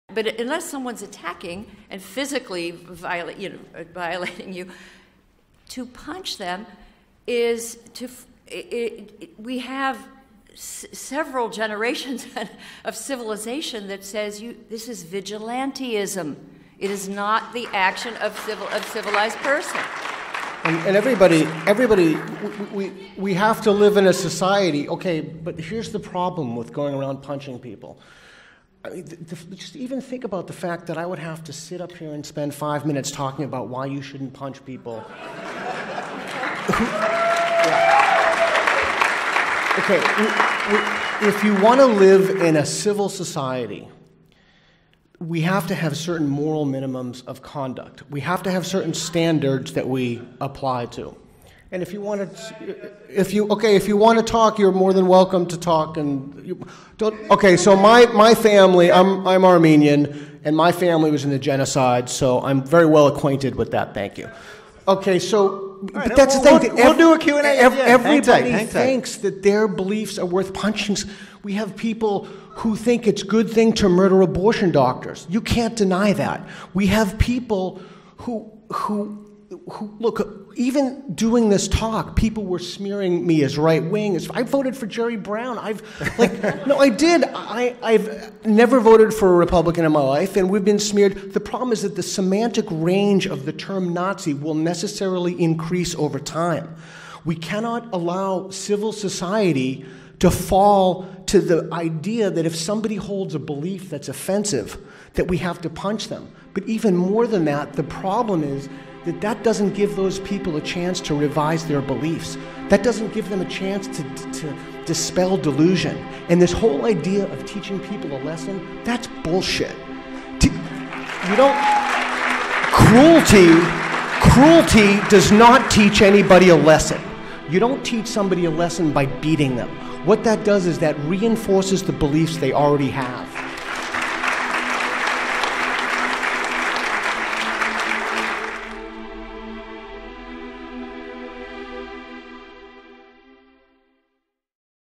Peter Boghossian on Punching Nazis
Peter Boghossian (Portland State University, @peterboghossian) explains why assaulting those who's ideas conflict with your own is bad for society.

Ghosted Liberty - A vehicle for free speech and civil discourse

@ghostedliberty on twitter